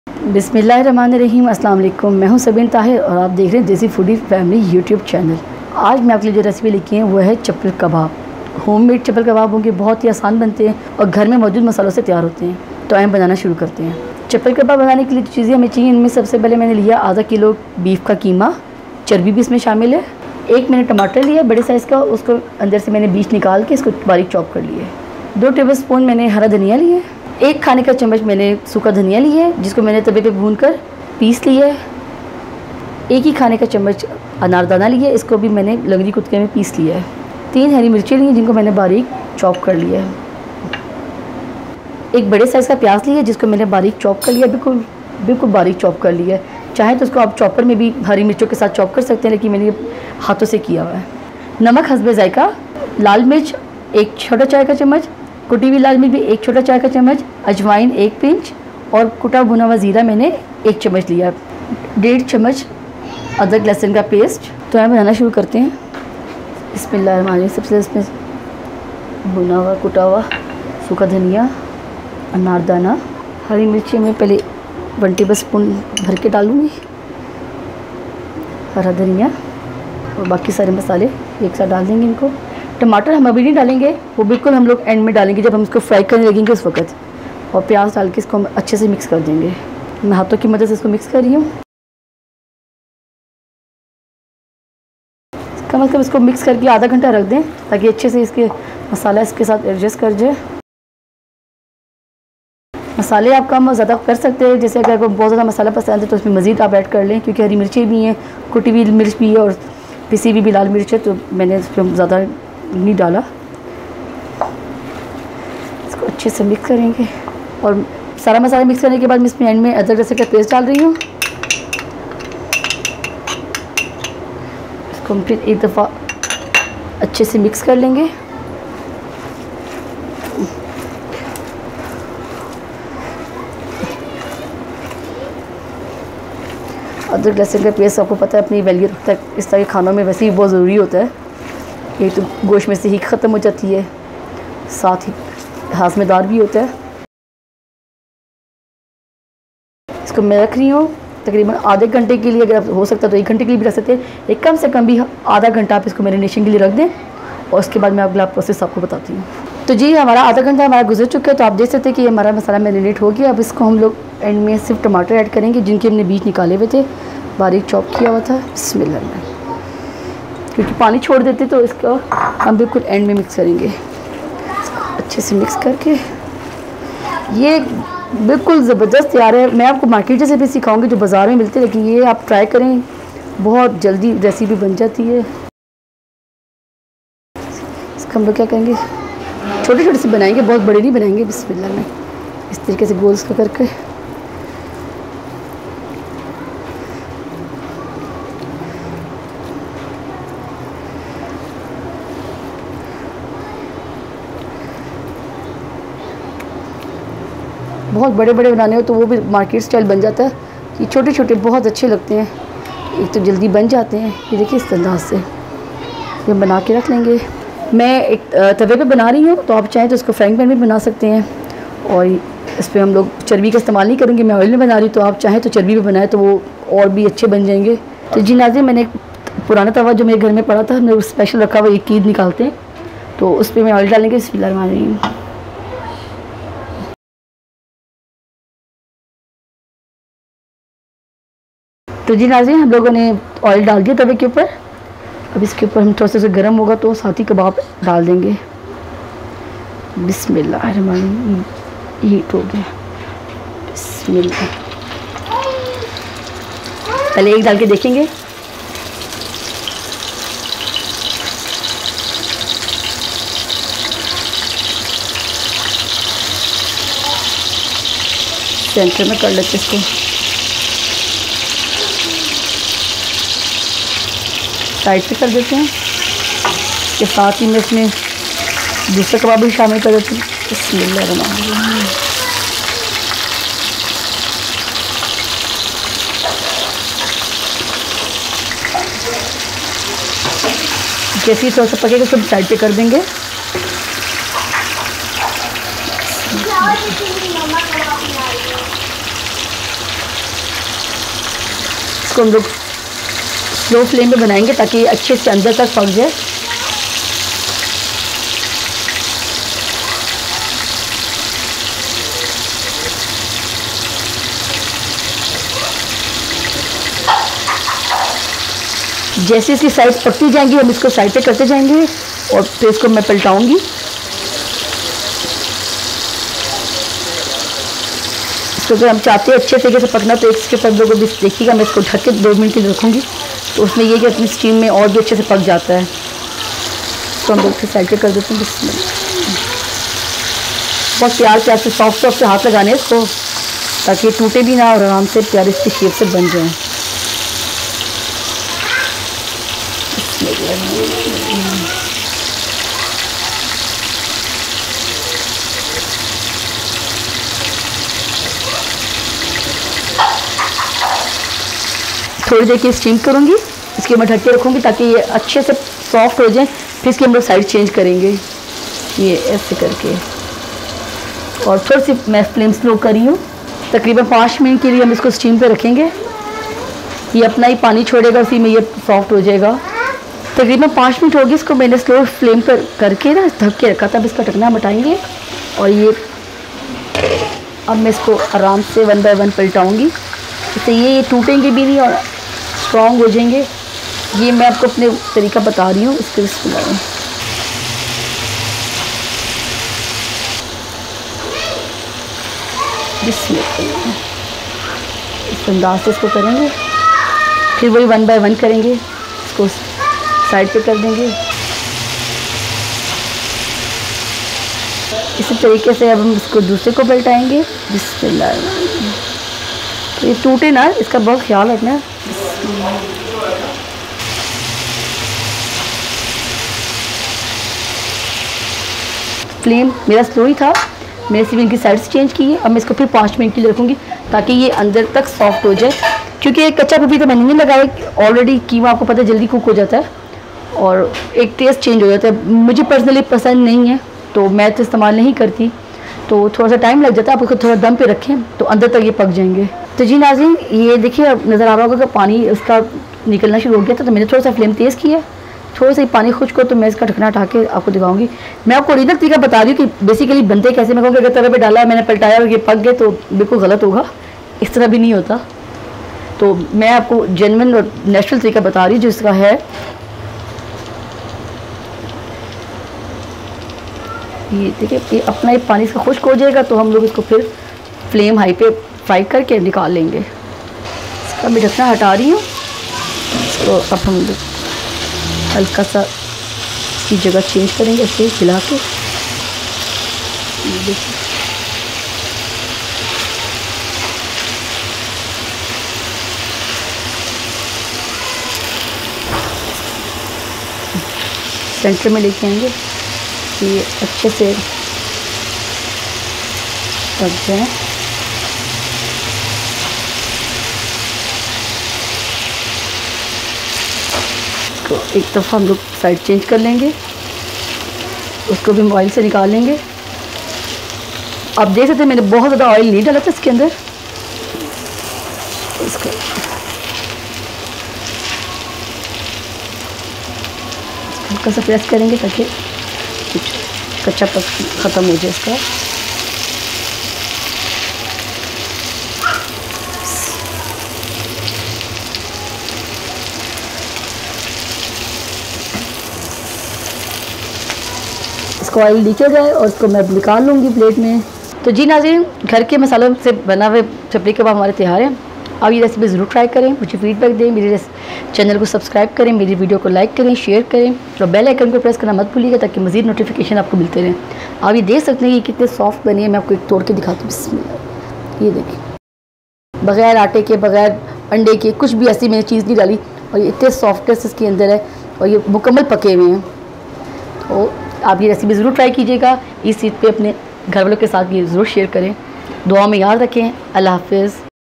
Chapli Kabab Recipe | Homemade Easy Chapli Kabab | Secret Of Chapli Kabab
How to make easy and quick homemade chapli kabab, step by step recipe.